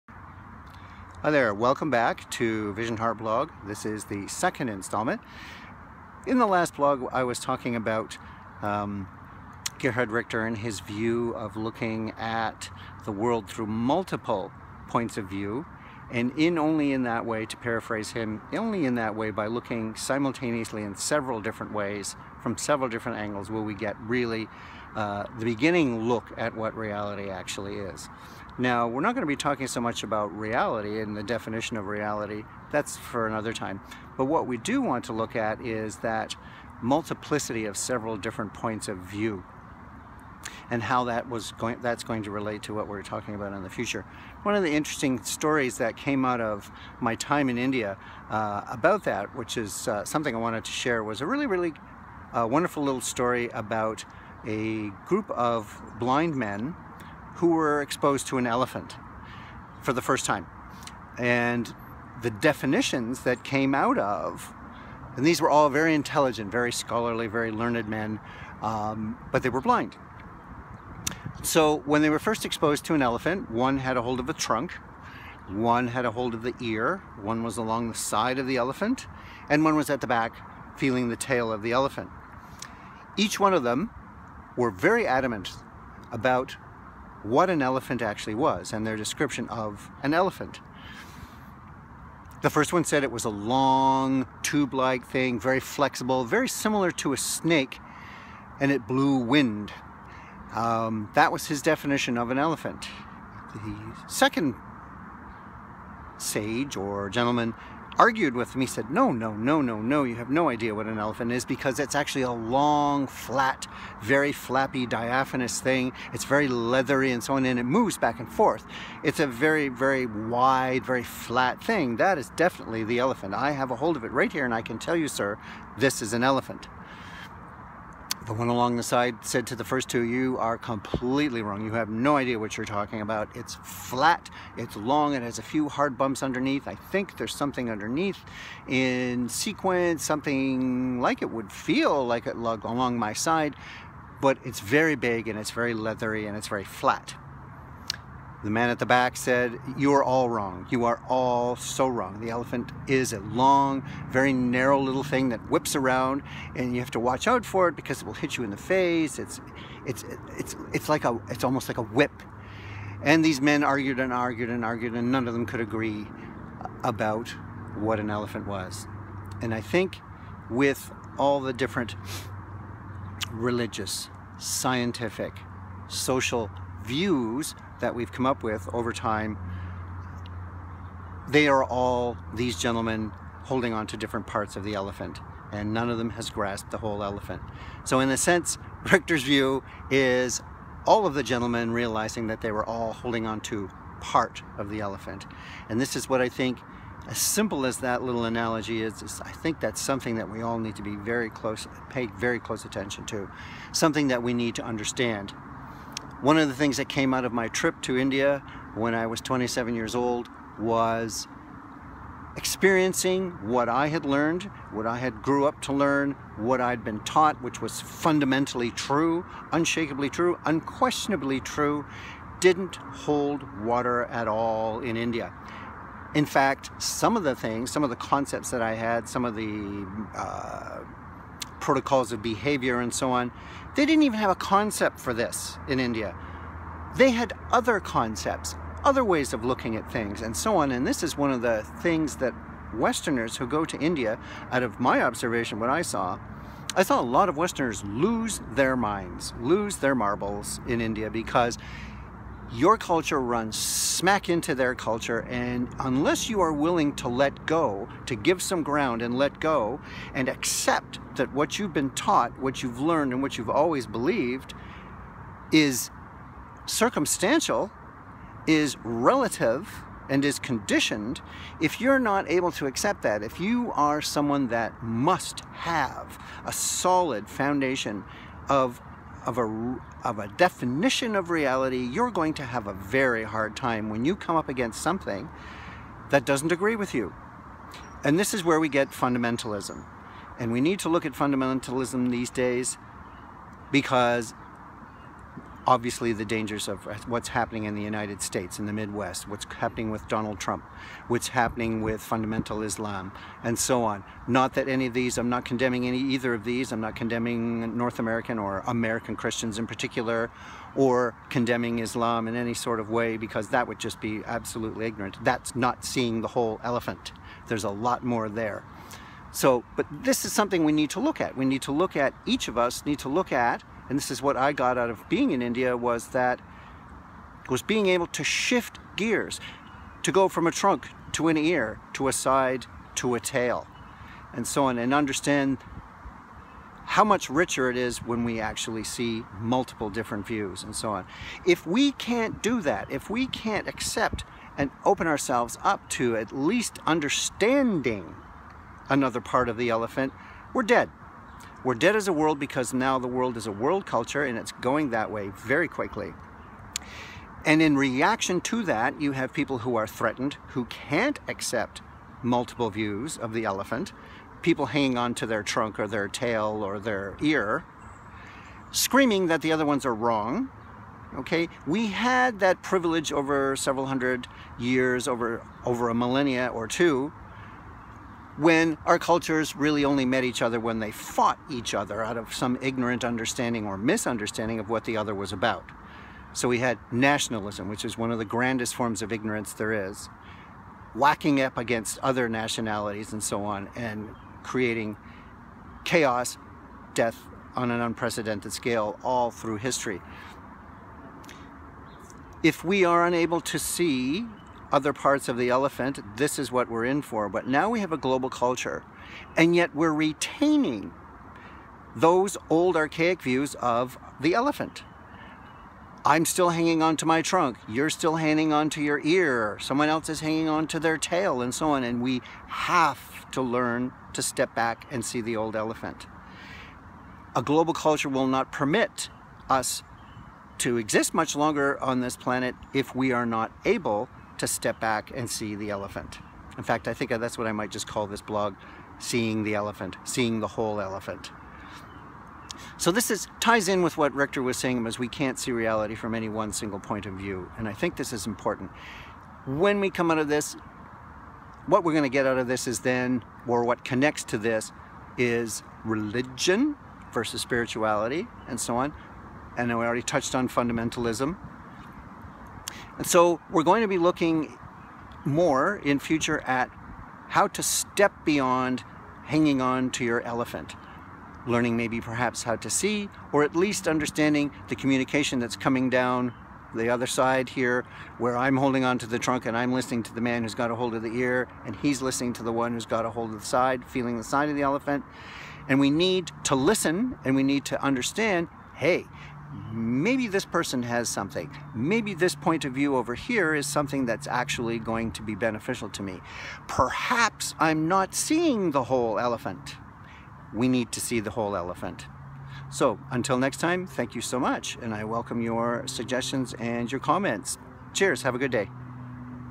2.) Seeing The Whole Elephant 1
By way of a metaphorical story from India, a continuing examination of seeing 'reality' from multiple points of view and beginning to understand the importance of doing so.